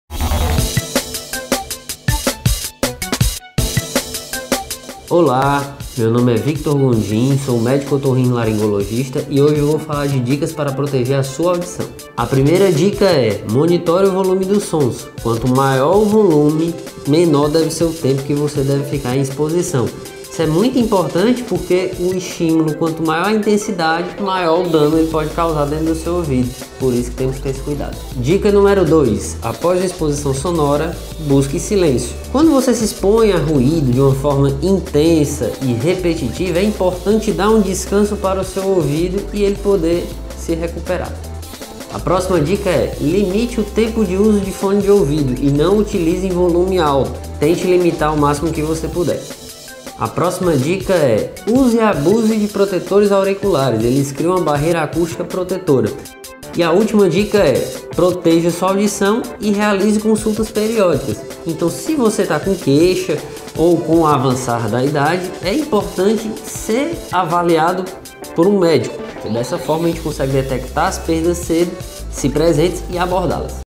DICAS Para PROTEGER A Sua AUDIÇÃO
Gosta de escutar música nos fones de ouvido? No vídeo de hoje, o otorrinolaringologista Victor Gondim apresenta algumas dicas para os cuidados com a audição. Confira!